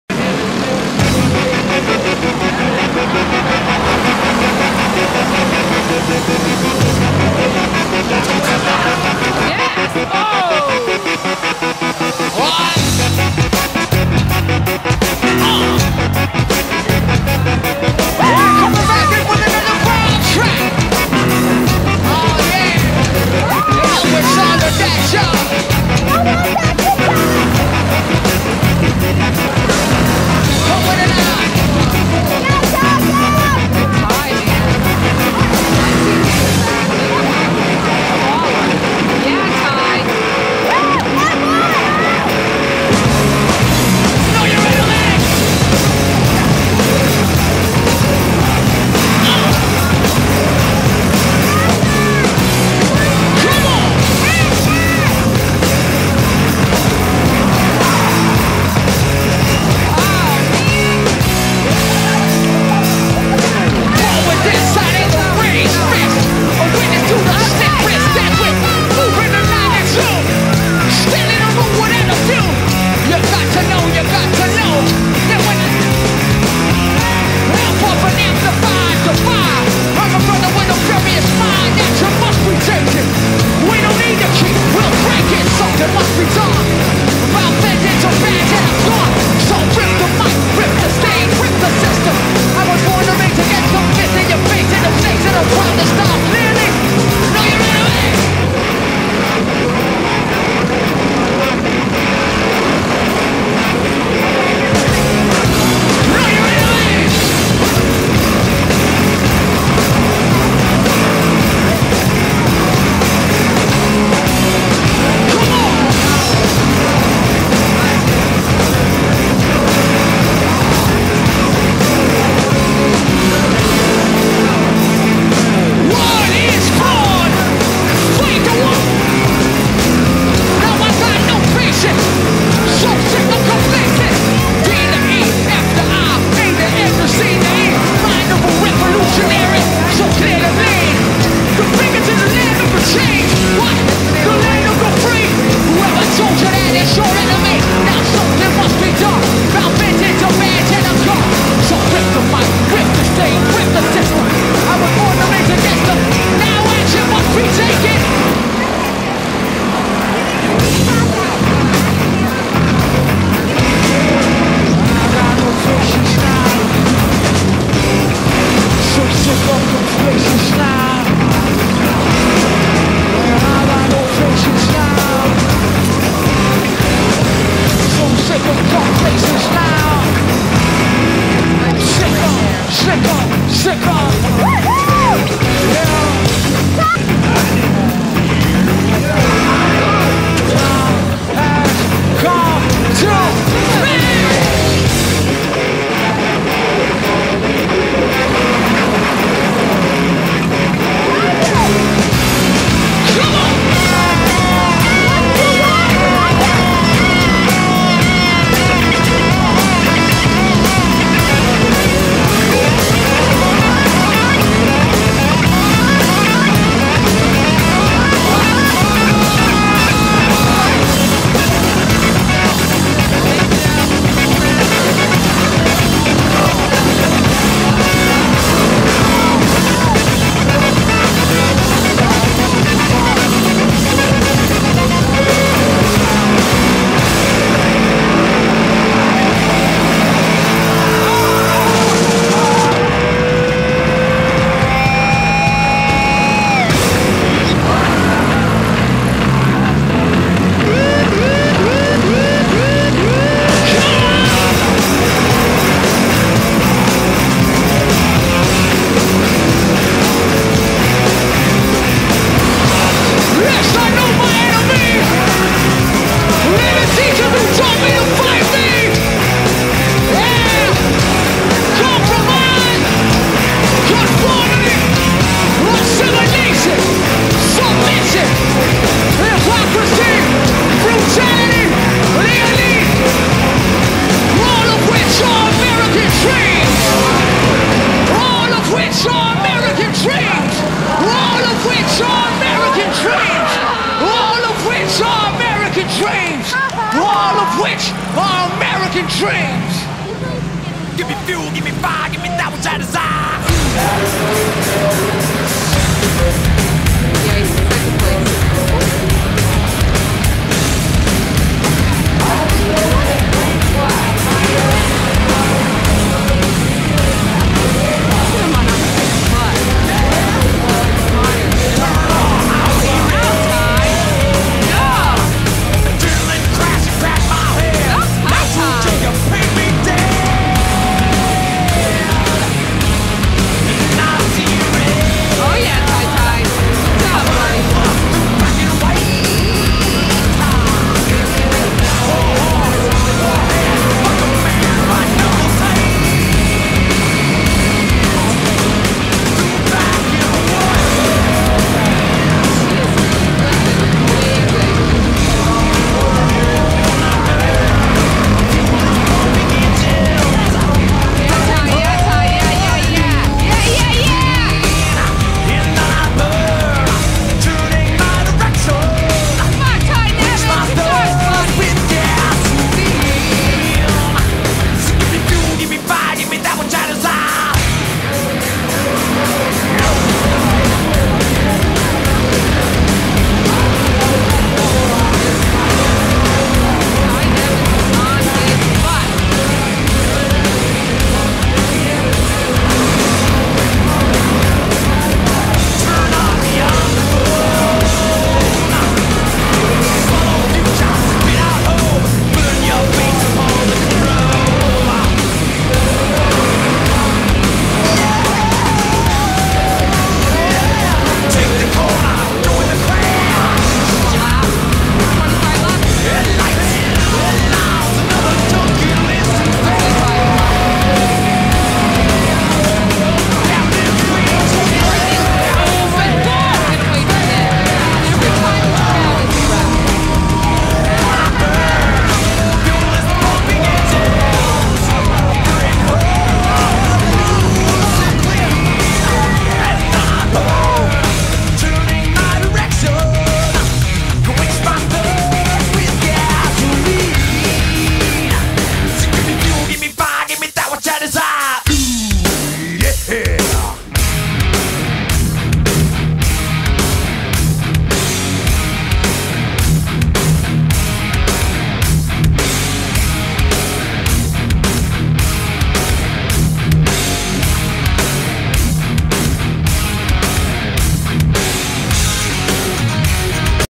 TY NEVINS JR HONDA NEW SMYRNA SPEEDWAY JUNE 10TH 2011.avi
Ty got wrecked in his heat race, the call went against the other driver, but Ty's car was broke so he had to pit. Would have restarted 1st, but because he had to pit, he was forced to restart last. Started last in the feature, raced up to 2nd...got in to it for the 3rd time with the same opponent - this time the judges called Ty for rough driving and threw him out of the race - bummer DNF.